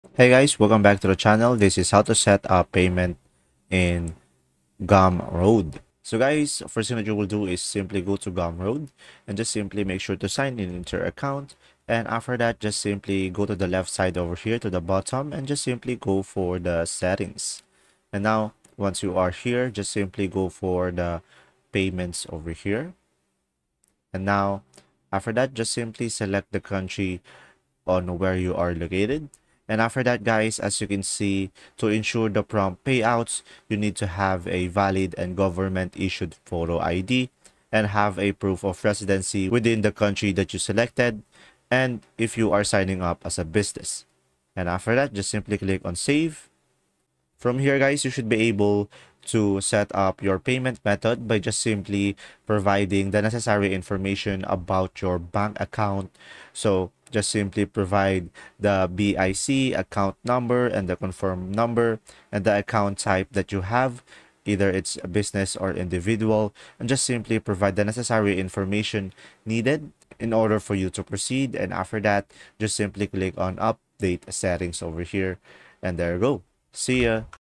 How to set up payment in Gumroad (Step By Step) 2025
How to set up payment in Gumroad (Step By Step)

Learn how to set up payment in Gumroad with this step-by-step tutorial! Whether you're selling digital products or trying to make money online, this guide will help you navigate the process easily!